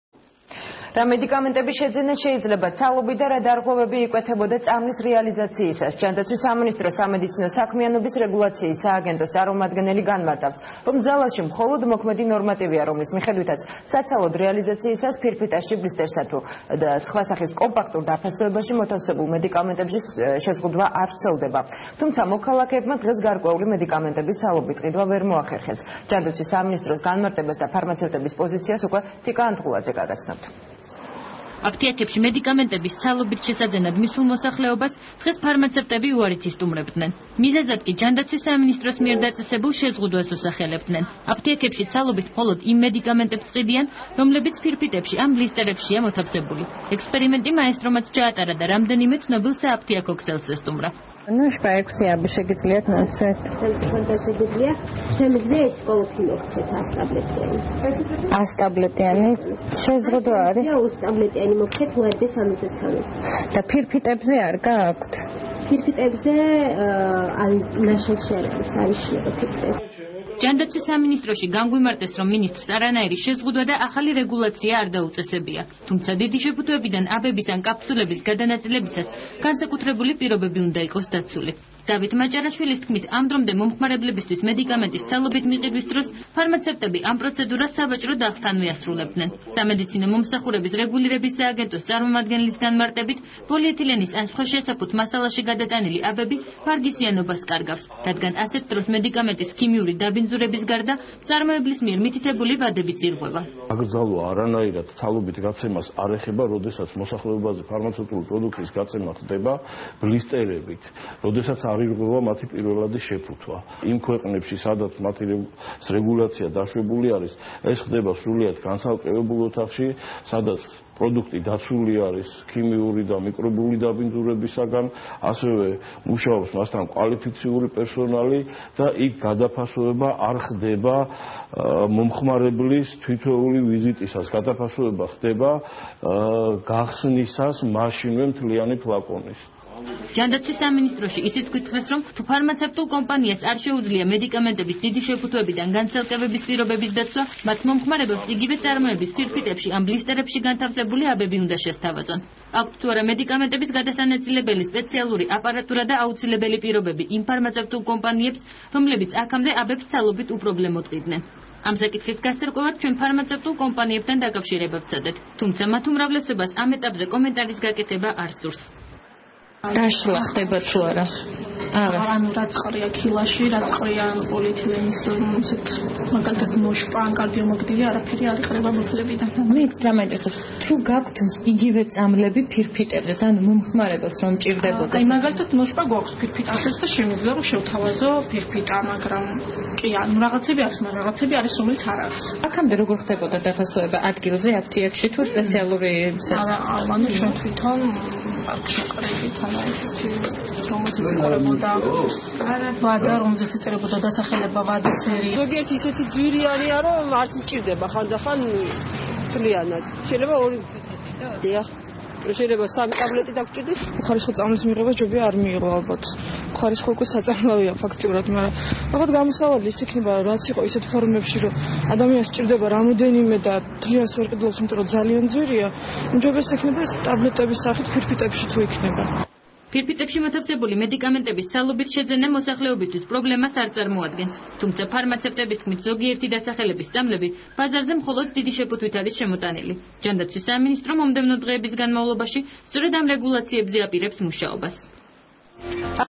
წამლების საცალო გაყიდვის შესახებ
შრომის, ჯანმრთელობისა და სოციალური დაცვის სამინისტრო განმარტავს სააფთიაქო ქსელებში ბოლო ხანებში მედიკამენტების საცალო გაყიდვის შეფერხების არსს:
არანაირი სახის ახალი რეგულაცია არ ყოფილა დაწესებული. ძალაშია მხოლოდ მოქმედი ნორმატივები, რომლის მიხედვითაც საცალო გაყიდვაზე არ ვრცელდება არანაირი სახის შეზღუდვა მაშინ, როდესაც მედიკამენტი მოთავსებულია ფირფიტაში (ბლისტერში) ან სხვა სახის კომპაქტურ პირველად შეფუთვაში, რომელიც დაშლას არ საჭიროებს (პაკეტი, ფლაკონი, ამპულა, კარტრიჯი და სხვა).
რაც შეეხება დიდი მოცულობის შეფუთვიდან აბების ან კაფსულების გადანაწილებას სხვა შესაბამის შესაფუთ მასალაში, ახალი შეზღუდვა ან რაიმე სახის აკრძალვა არც აქ ყოფილა დაწესებული. განსხვავება ის არის, რომ ამ სახის გადაფასოება (განსხვავებით ზემოთ აღწერილი განცალკევებისაგან) სააფთიაქო პირობებში მოითხოვს გარკვეული პირობების დაცვას, რომელიც უახლოეს პერიოდში მოექცევა რეგულაციაში და მათი საცალო გაცემა შესაძლებელი გახდება შეუფერხებლად.
იმ მოქალაქეებს, რომლებსაც პრობლემები შეექმნება მედიკამენტების საცალო შეძენაზე, შეუძლიათ დაუკავშირდნენ ჯანდაცვის სამინისტროს ცხელ ხაზს ტელეფონზე: 15 05